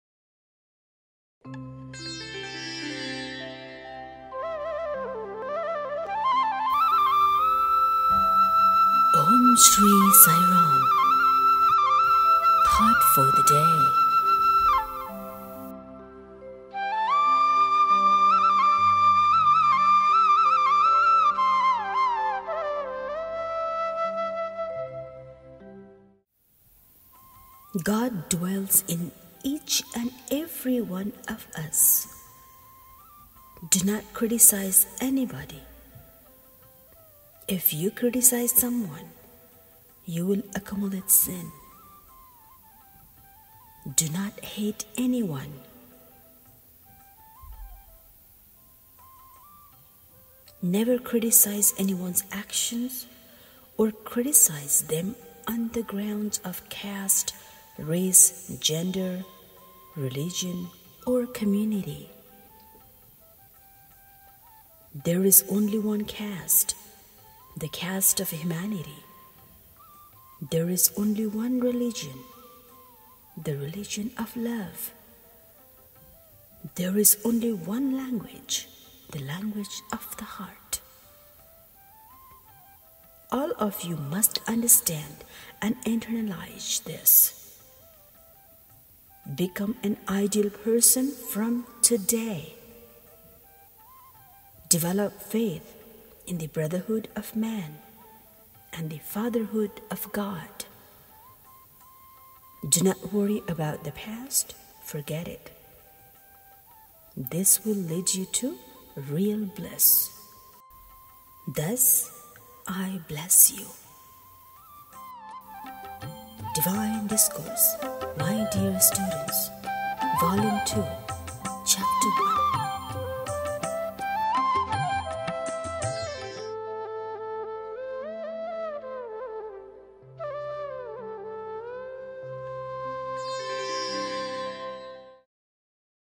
Thought for the day as written in Prashanti Nilayam on 16-Feb-2014
THOUGHT FOR THE DAY Date: Sunday, February 16, 2014
God dwells in each and every one of us. Do not criticise anybody. If you criticize someone, you will accumulate sin. Do not hate anyone. Never criticise anyone's action or criticize them on the grounds of caste, race, gender, religion or community. There is only one caste, the caste of humanity. There is only one religion, the religion of love. There is only one language, the language of the heart. All of you must understand and internalize this. Become an ideal person from today. Develop faith in the 'Brotherhood of Man and the Fatherhood of God'. Do not worry about the past, forget it. This will lead you to real bliss. Thus I bless you! (Divine Discourse, "My Dear Students", Vol 2, Chapter 1)-BABA

  Read by                  Seema
 Video & Graphic Art      Gajendra sairam

"Videos for Thought for the day are made by children of Sai"
(Thought for the day Team)

"Sharing  teachings of Bhagwan Sri Sathya Sai Baba"
       for Universal love & Peace
             Jay Sairam...!!!